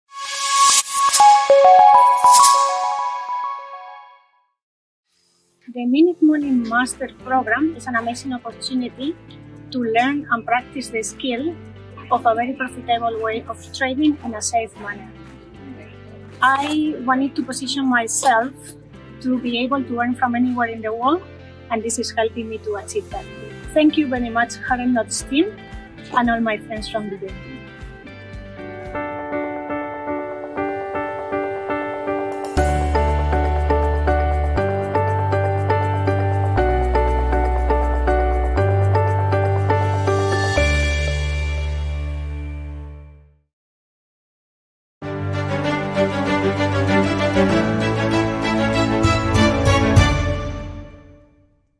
Member Review: Isabel on the Hodlnots 3M Program
Our Hodlnots member Isabel tells about her experience with the Hodlnots 3M Program. She share how it helped her position herself.